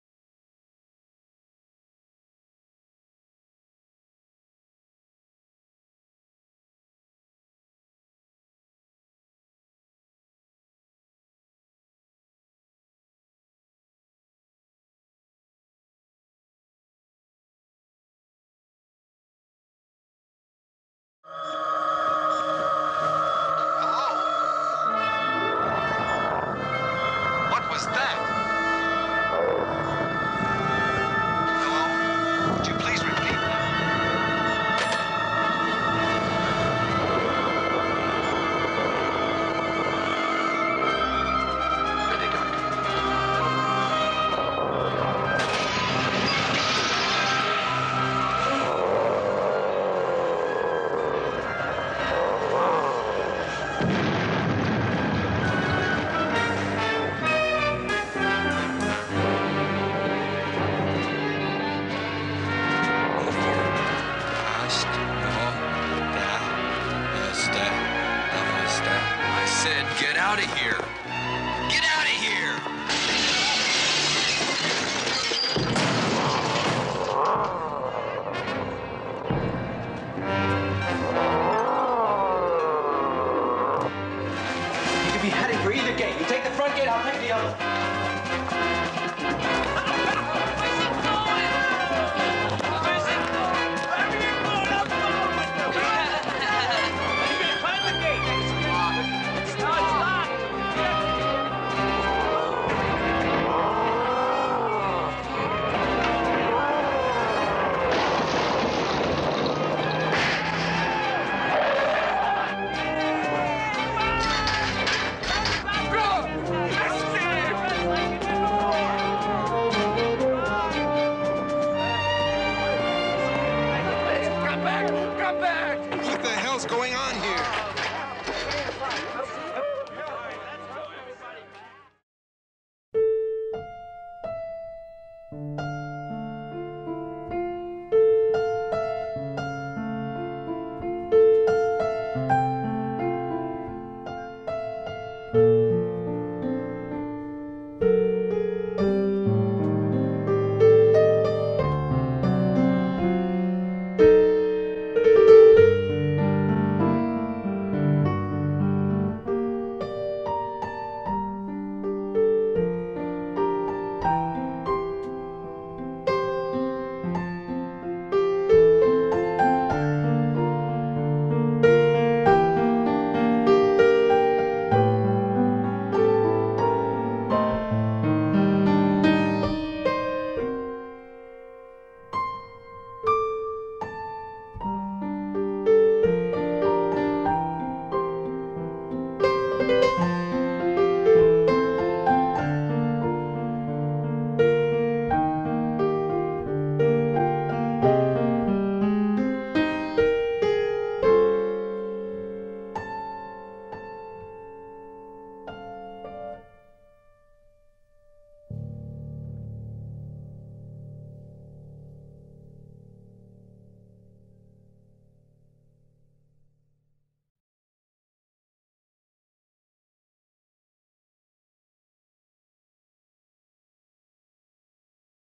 Martin O'Neill resigning - EXCLUSIVE FOOTAGE
Having received a phone call from Randy Lerner informing him that he must sell James Milner, Martin O'Neill loses his temper and goes on the rampage.